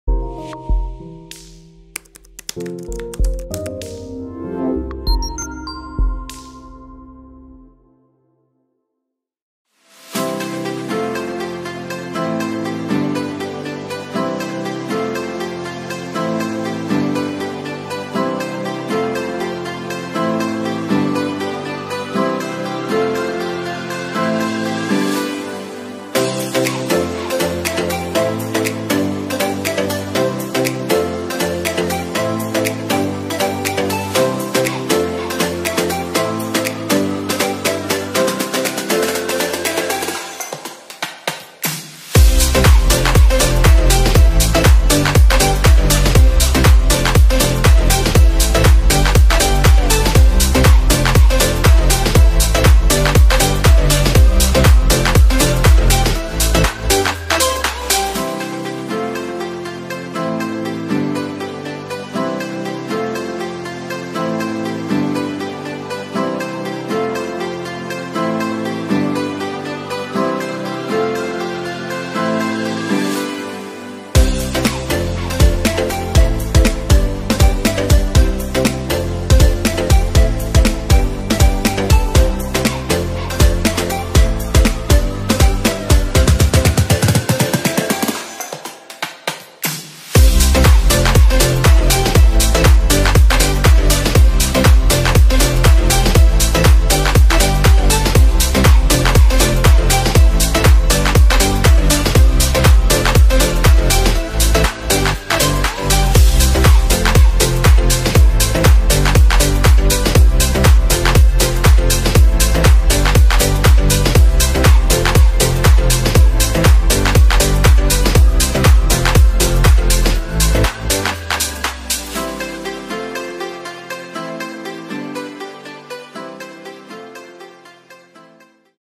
2013 Bailey Unicorn Seville
Take a look around this 2013 Bailey Unicorn Seville, currently for sale at Highbridge Caravans.